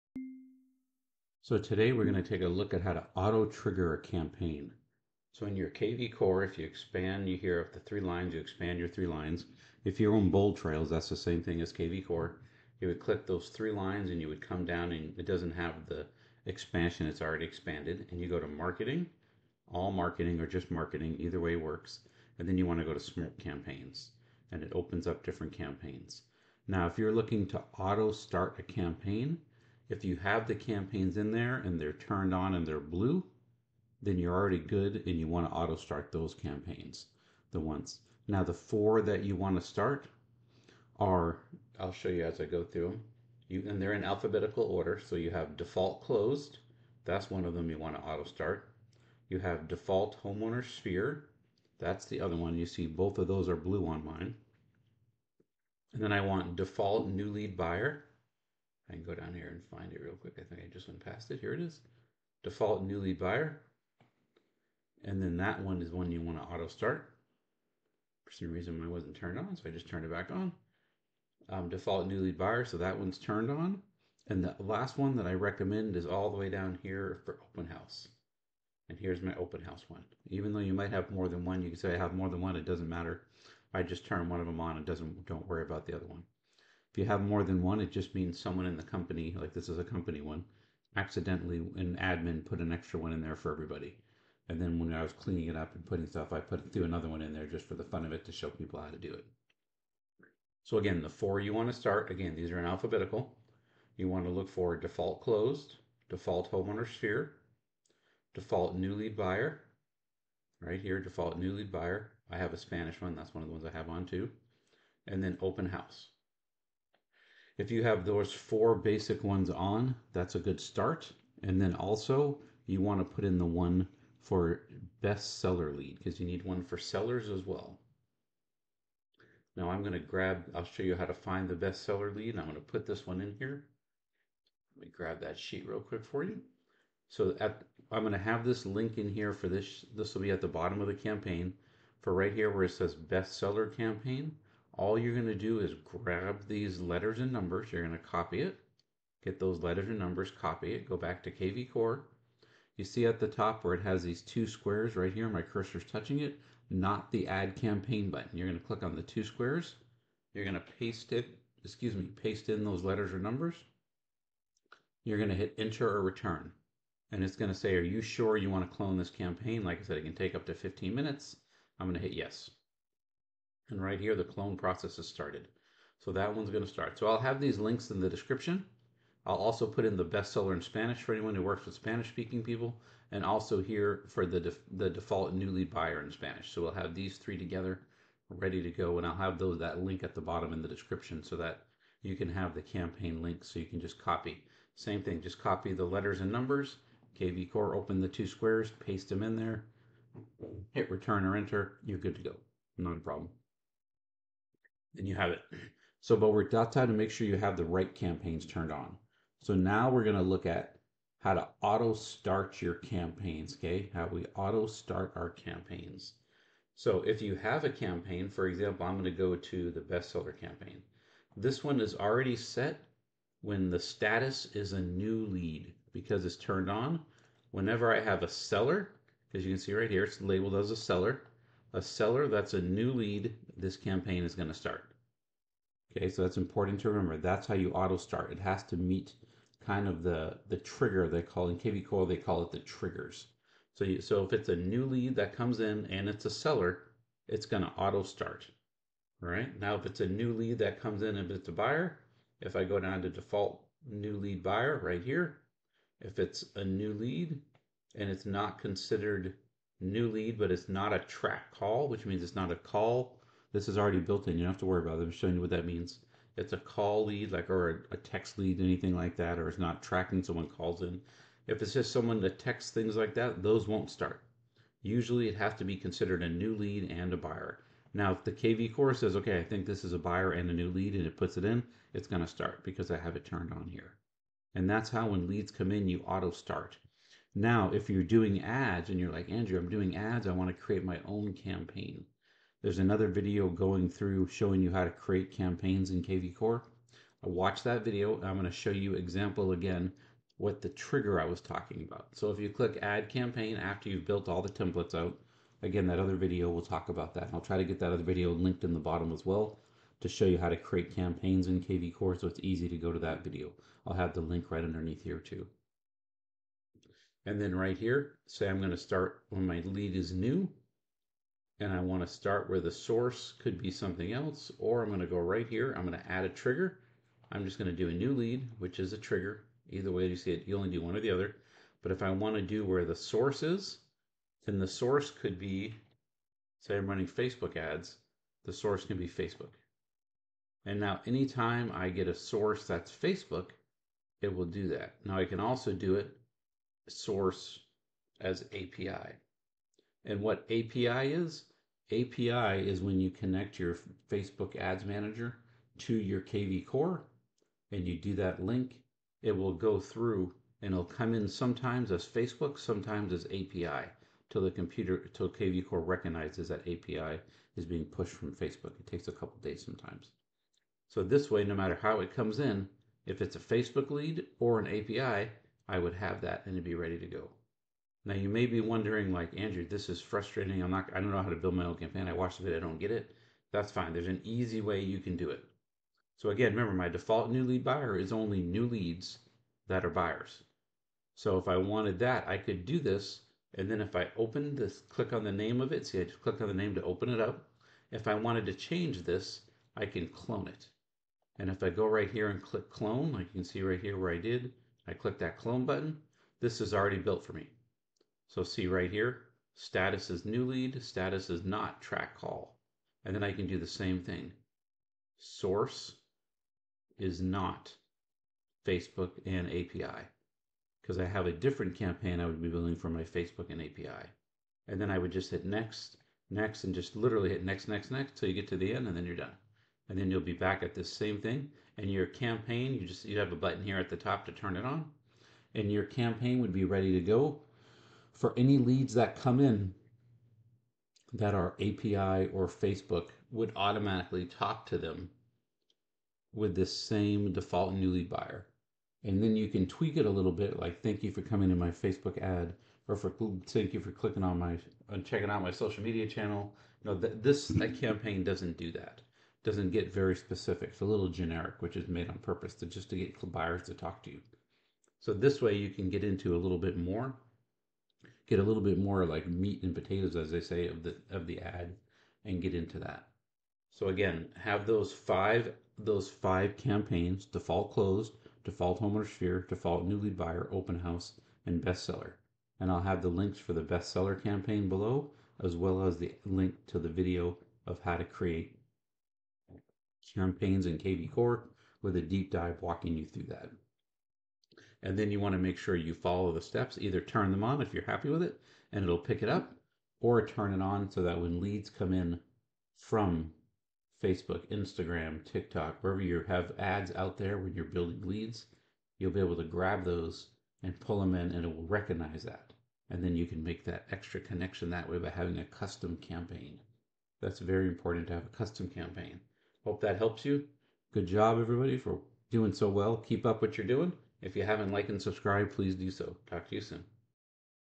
AutoStart Campaigns in KvCORE and BoldTrail
Learn how to auto-start campaigns in KvCORE and BoldTrail. Also which campaigns you should have turned on to be most effective.

Links to pre-made campaigns:
Best seller campaign
a68634c8-85da-48b4-81d0-8865aa2d2579

The best seller in (Spanish)
121a644e-bdca-4e29-a392-6fca4f0ce8bf

Default new lead  - Buyer (Spanish)
153850c2-bff1-403a-a5c7-2fa5ef0ab63d